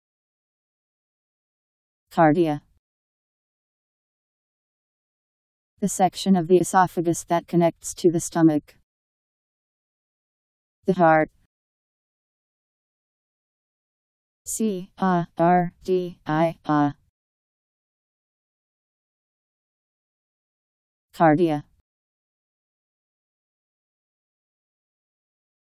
Cardia Meaning
Video shows what cardia means. The section of the esophagus that connects to the stomach.. The heart..  Cardia Meaning. How to pronounce, definition audio dictionary. How to say cardia. Powered by MaryTTS, Wiktionary